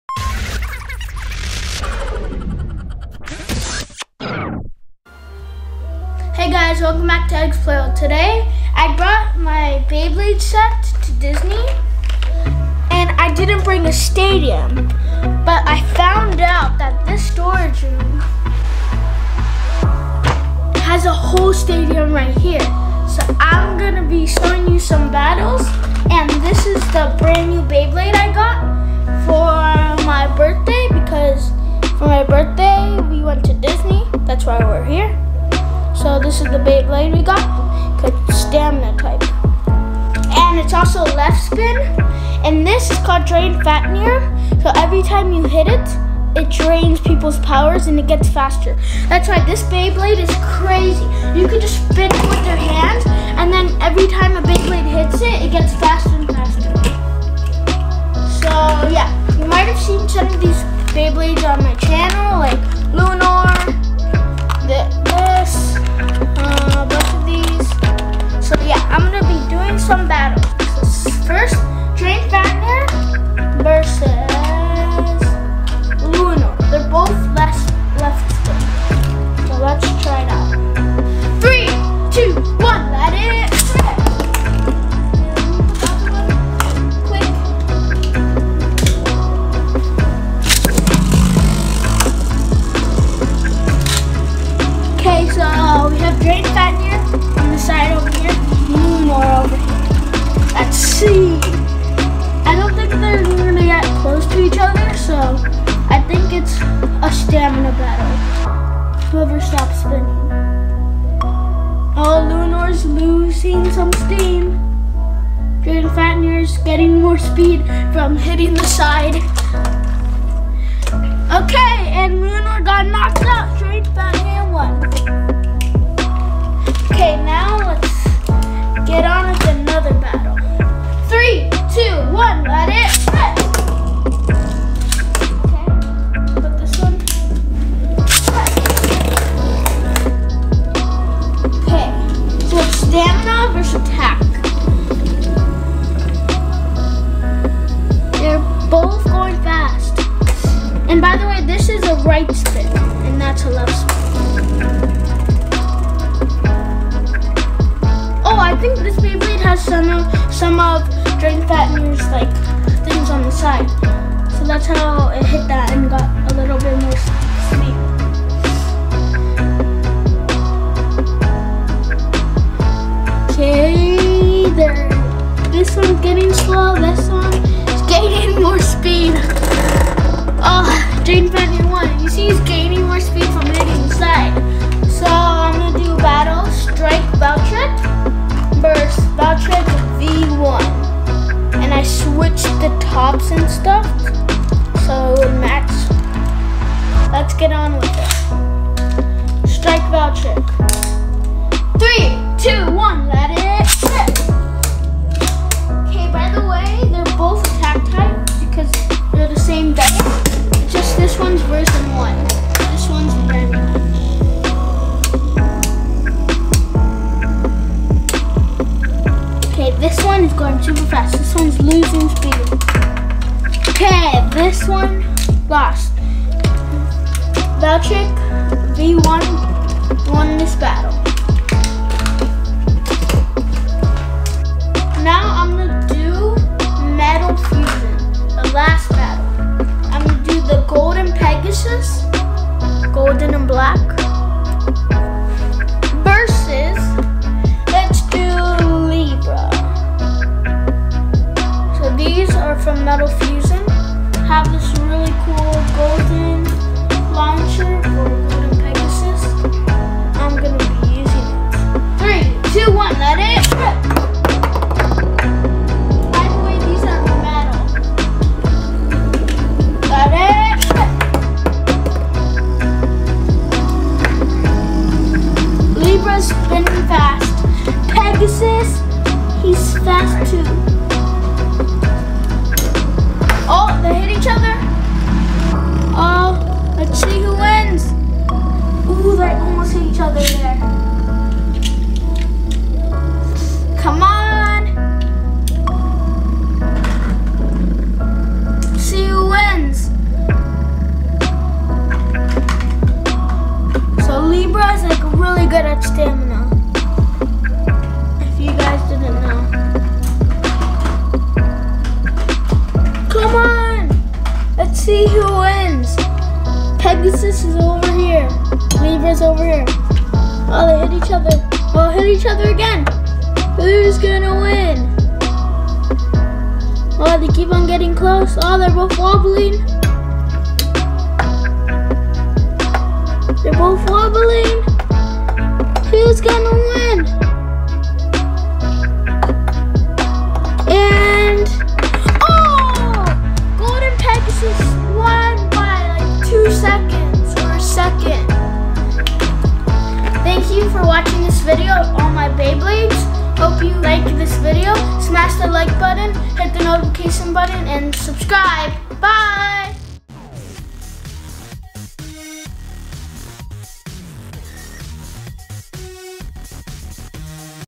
Awesome Beyblade Battles!! || Drain Fafnir
Took some of my Beyblades to Disney but didn't take my stadium. Instead, I used the storage bin in the room and it worked out nicely!!